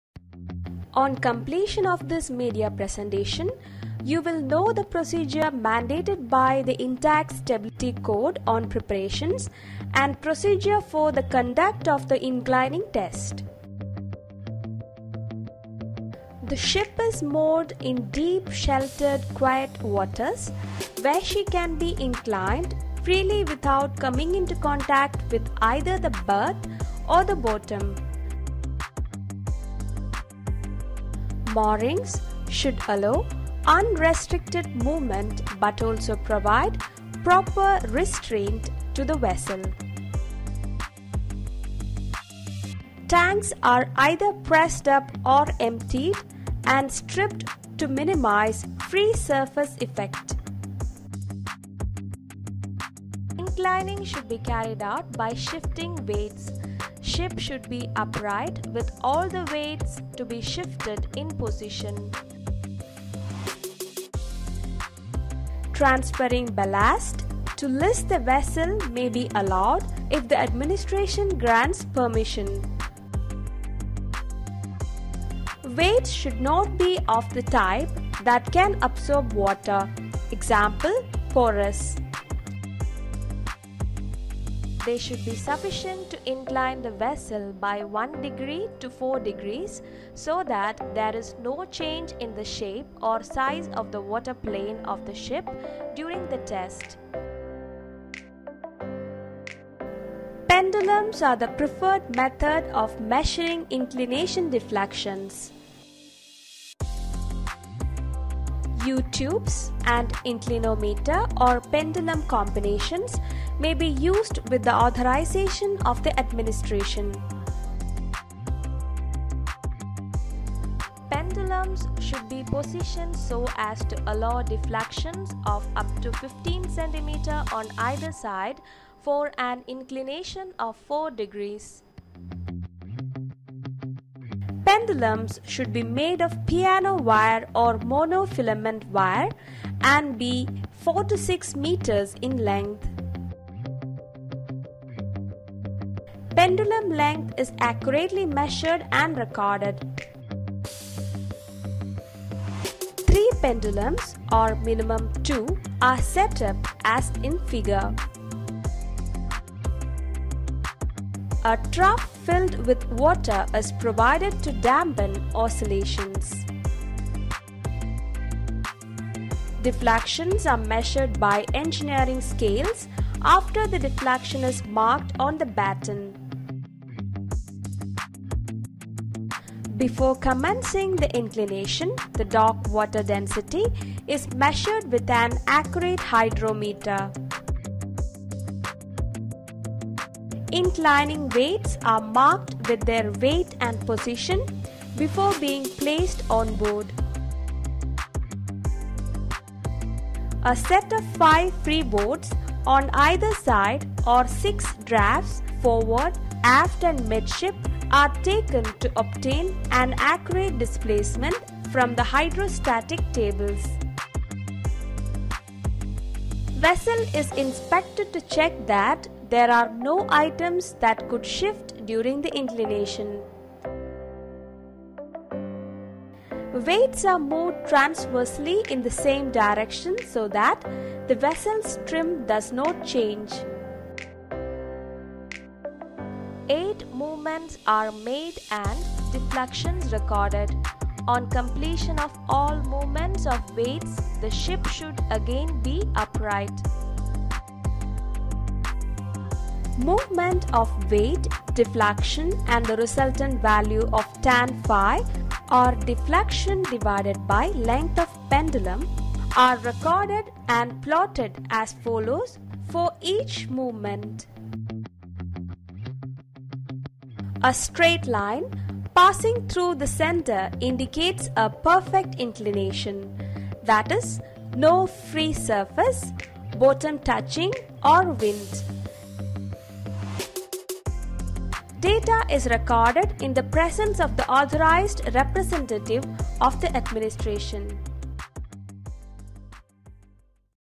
HOW TO PERFORM INCLINATION EXPERIMENT | SHIP STABILITY | NAVAL TERM | SAFETY MEO CLASS 4 ORALS
SAFETY ORAL

Thanks and regards to with credit to DGMARINE
Music credit to APOLLO PATRIK